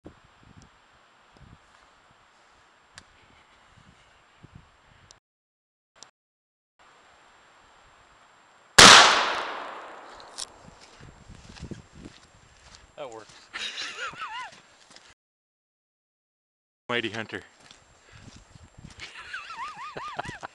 friend shoots grouse
Friend shoots grouse with shotgun